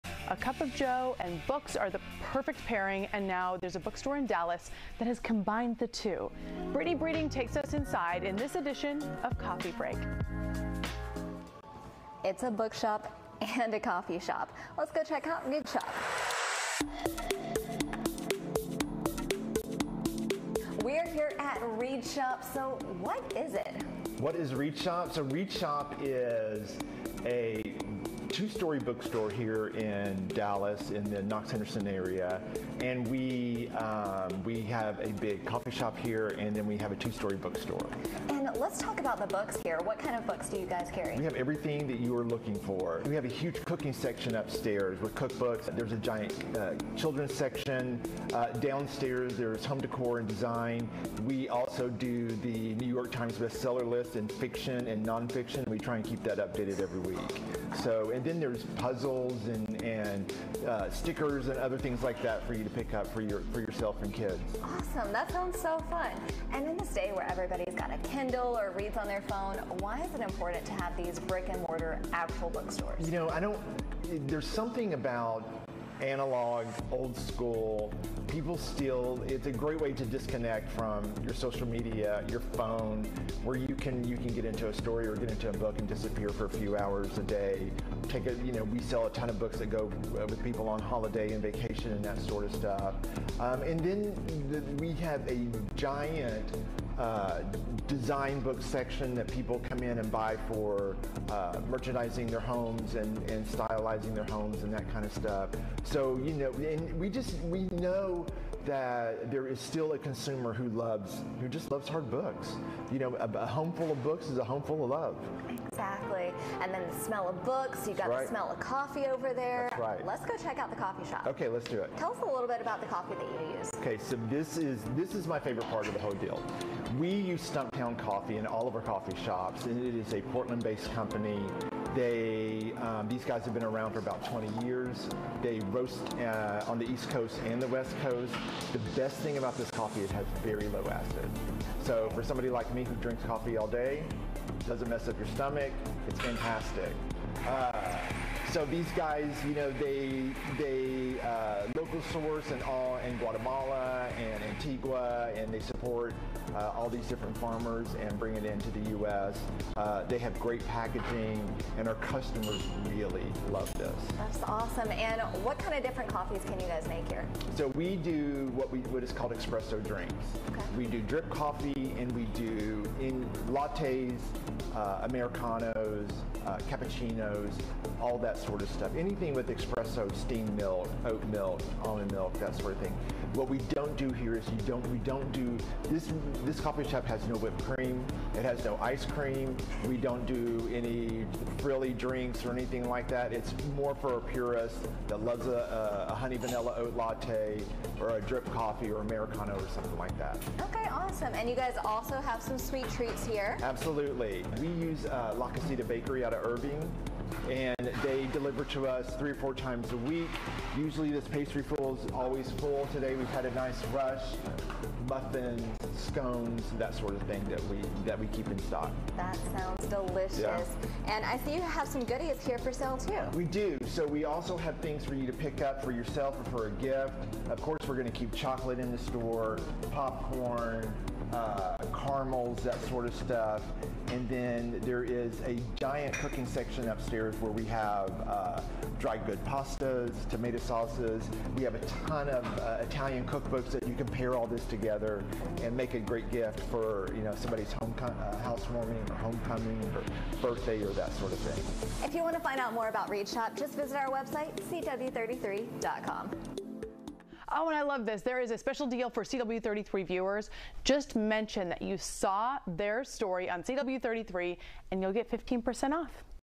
Relax at a coffee shop, bookshop all in one
Calling all book-lovers, there’s a two-story bookshop with a coffee shop inside and it’s right here in Dallas.